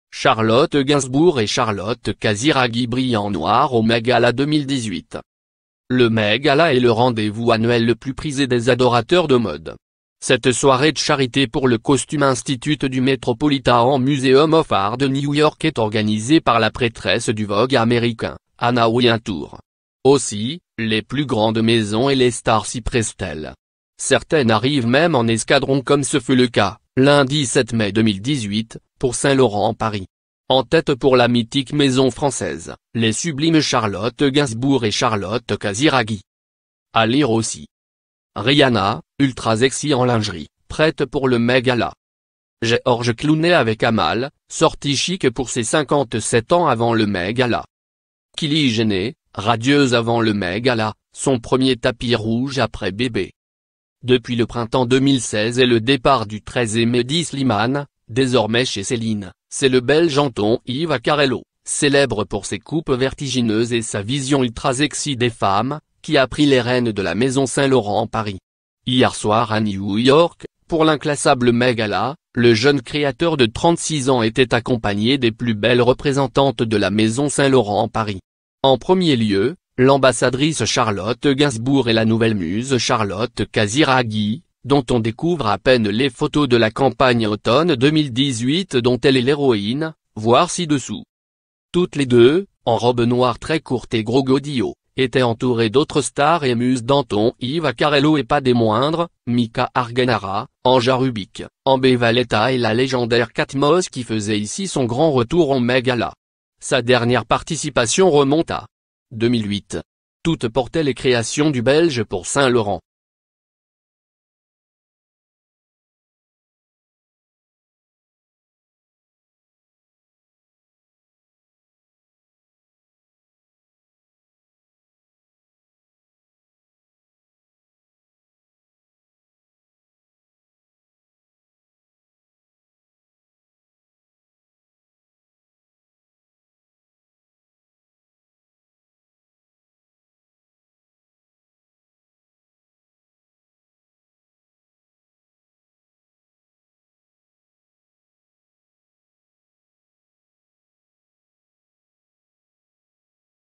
Charlotte Gainsbourg et Charlotte Casiraghi brillent en noir au Met Gala 2018
Le Met Gala est le rendez-vous annuel le plus prisé des adorateurs de mode. Cette soirée de charité pour le Costume Institute du Metropolitan Museum of Art de New York est organisée par la prêtresse du Vogue américain, Anna Wintour. Aussi, les plus grandes maisons et les stars s'y pressent-elles... Certaines arrivent même en escadron comme ce fut le cas, lundi 7 mai 2018, pour Saint Laurent Paris. En tête pour la mythique maison française, les sublimes Charlotte Gainsbourg et Charlotte Casiraghi.